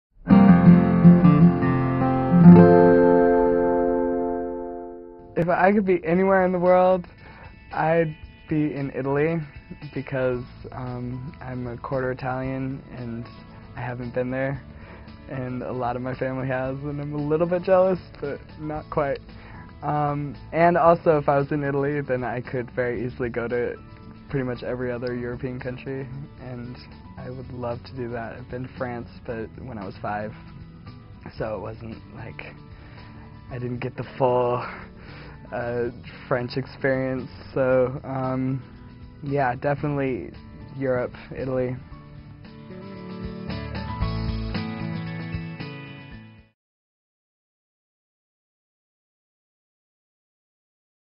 Singer Sanjaya Malakar: Anywhere
Learn more about Dancing to the Music in My Head if Sanjaya could be anywhere on this earth, where would he be?